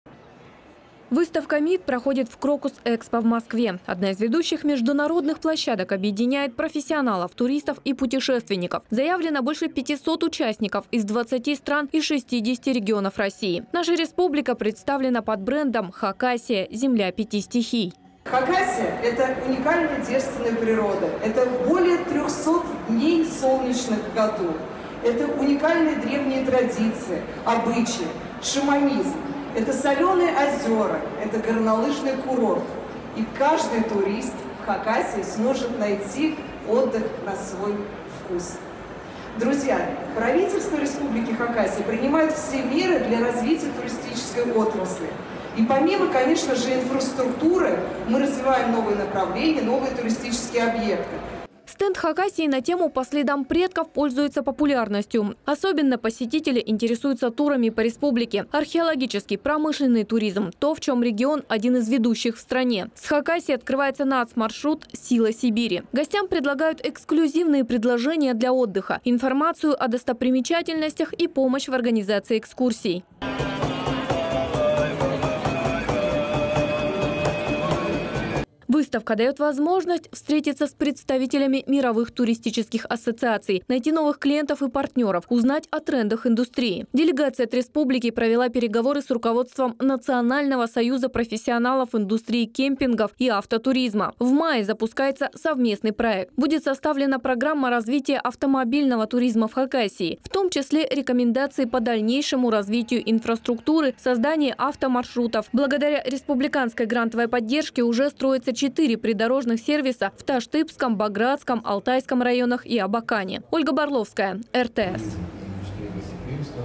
Хакасия - партнер и участник международной выставки туризма и индустрии гостеприимства
О солнечной Хакасии должен узнать весь мир. Наша республика стала партнёром и участником "MITT". Это международная выставка туризма и гостеприимства, которая проходит в Москве. Туристические возможности Хакасии представлены посетителям, турбизнесу, делегациям из других регионов и стран.

Подробности - в репортаже телеканала РТС.